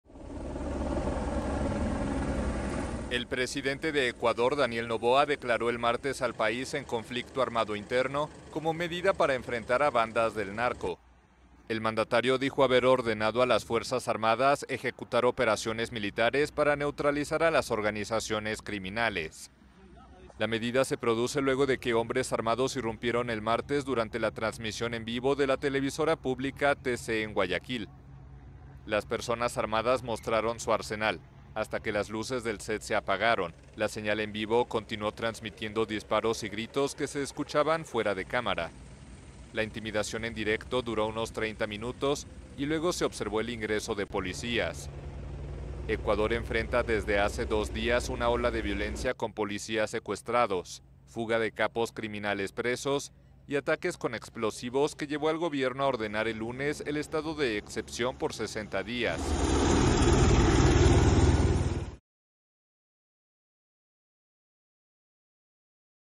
Ecuador declara "conflicto armado interno" tras irrupción de hombres armados en TV pública | AFP
Un grupo de encapuchados, armados con fusiles y granadas, irrumpió el martes durante la transmisión en vivo de la televisión pública TC en Guayaquil, en una nueva arremetida del narco que sacude a Ecuador desde hace dos días.